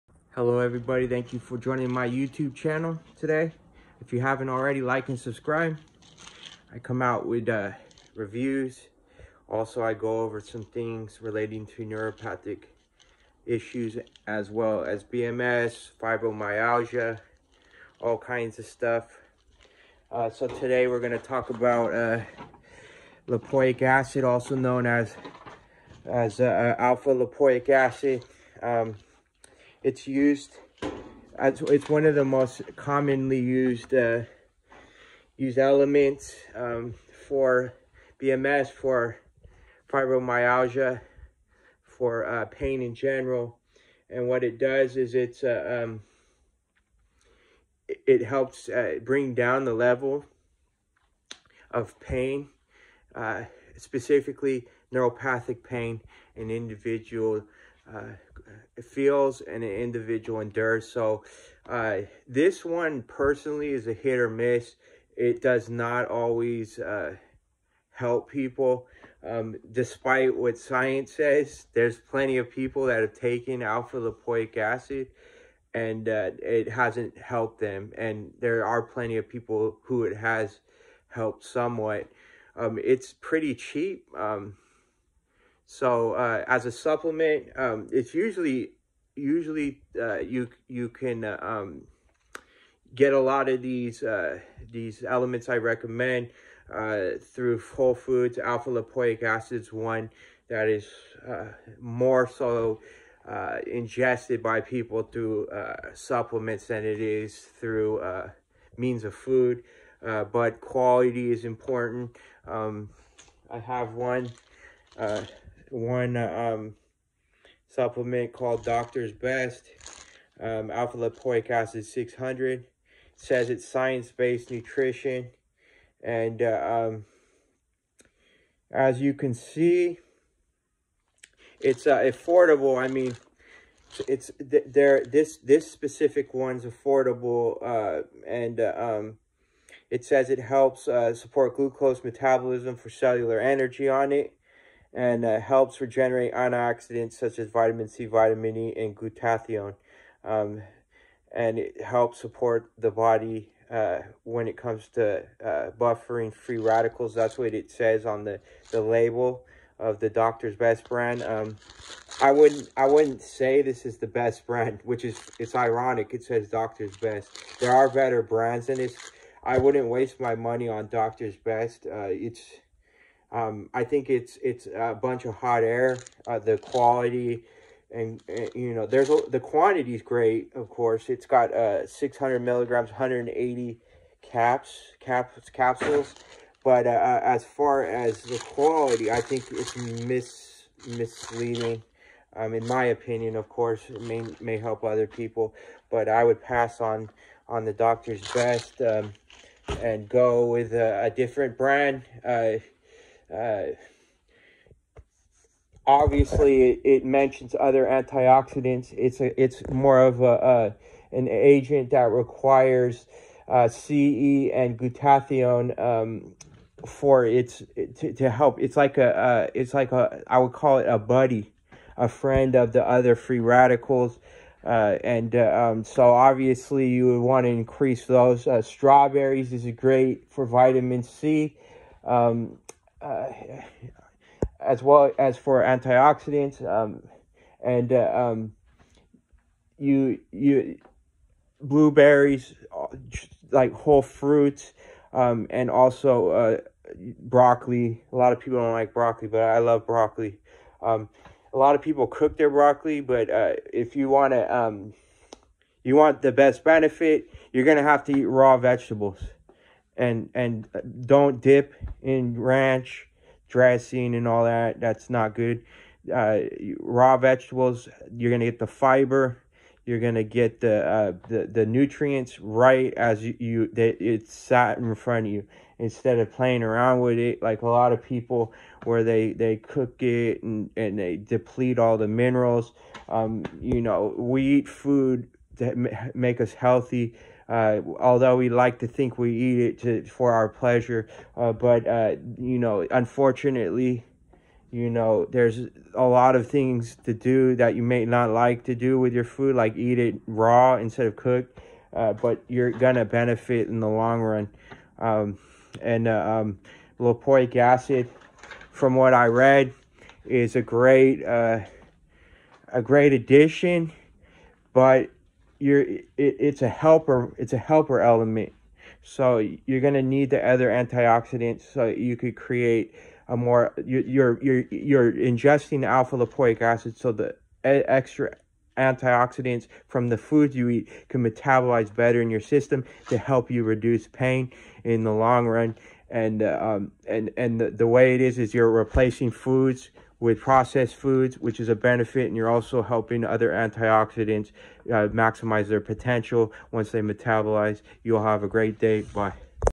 Burning Mouth Syndrome (BMS) and Alpha Lipoic Acid
Used for many nerve pain issues, APA, or Alpha Lipoic Acid is used to reduce pain by acting a a helper-element for antioxidants such as vitamins C and E. By helping combat free-radicals, APA is said to reduce pain caused by nerve damage and inflammation (some inflammation is not noticeable by the naked eye, or even a magnifying scope).